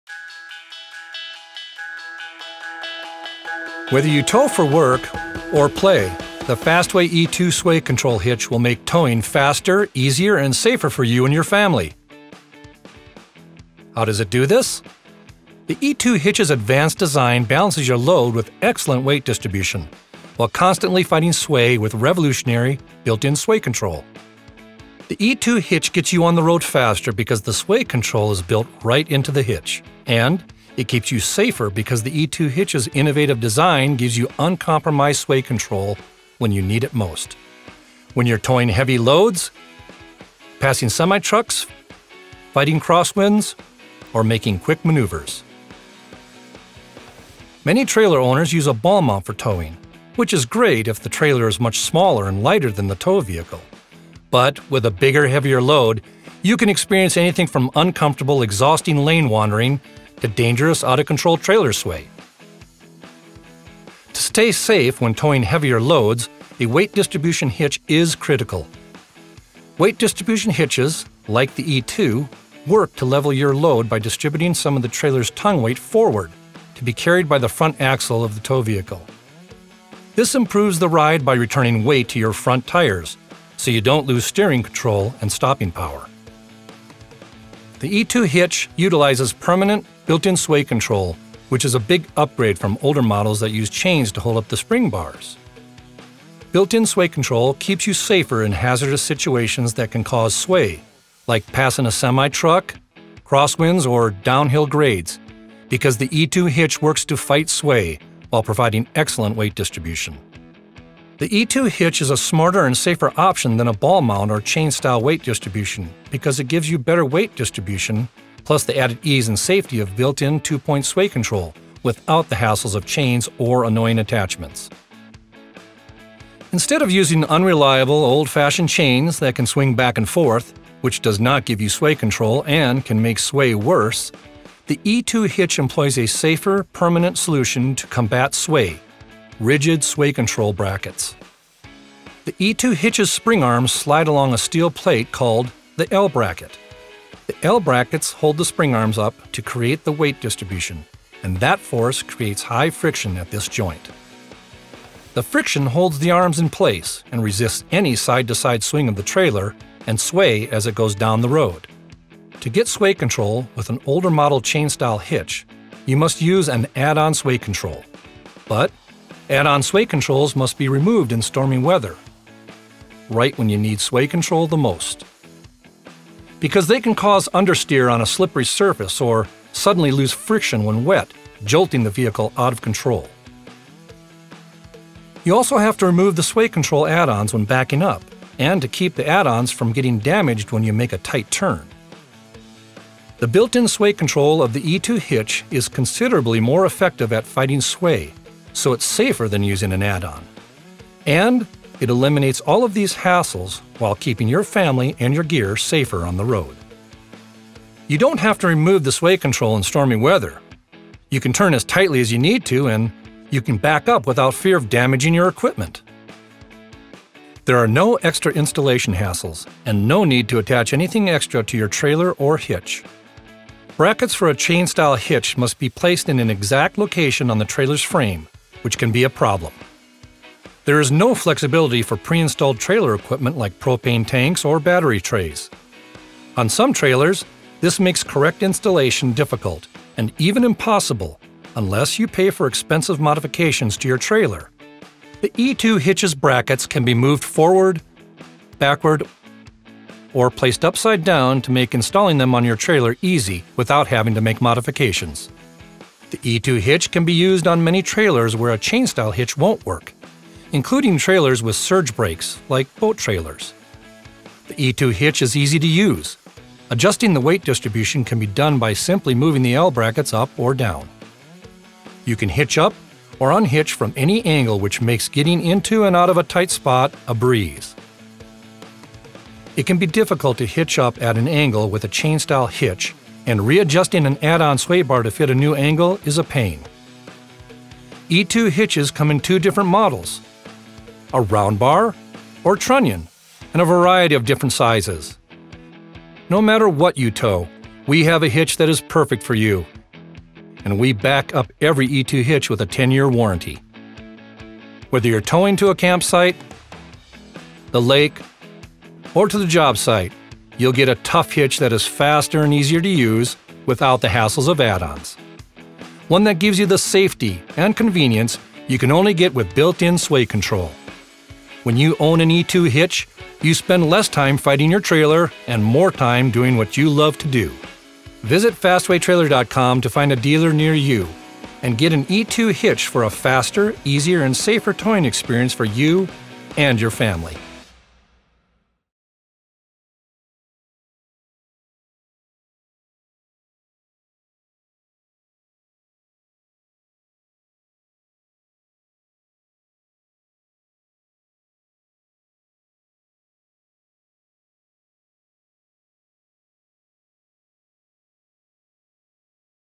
The Fastway e2 Hitch - A Faster, Easier and Smarter Way To Tow Your Trailer
When you're towing equipment to the job site, toys to play, or your camp trailer for a fun family getaway, you need the added convenience and protection of the Fastway® e2™ hitch's built-in sway control.

Its patented design gives you weight distribution and sway control all in one, without the hassles of extra work that come with add-on sway bars - the traditional way of getting sway control. With an e2 hitch, there is nothing extra to hook up, undo, or store. You can back up anytime you need to, as tightly as you need to, and never have to get out to take off the add-on to keep it from getting damaged.

Spend less time fighting your trailer equipment, and more time doing what you love to do with an e2 sway control hitch.